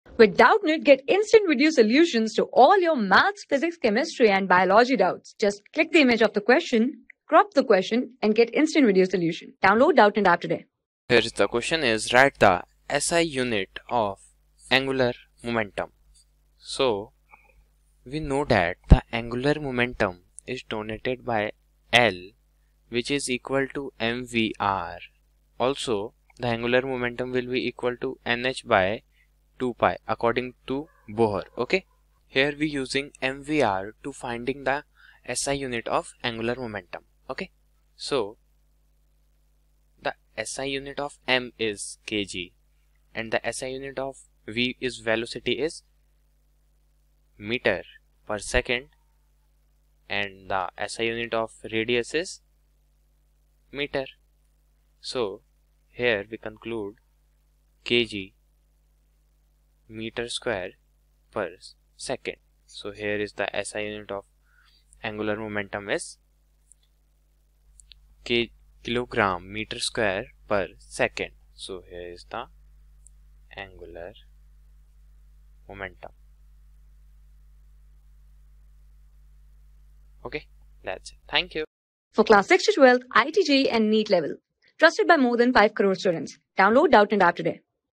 Write SI unit of angular momentum: | 12 | UNITS AND MEASUREMENTS | PHYSICS | AAKASH INSTITUTE EN...
Write SI unit of angular momentum:
Class: 12
Subject: PHYSICS
Chapter: UNITS AND MEASUREMENTS
Board:IIT JEE

👉 Have Any Query? Ask Us.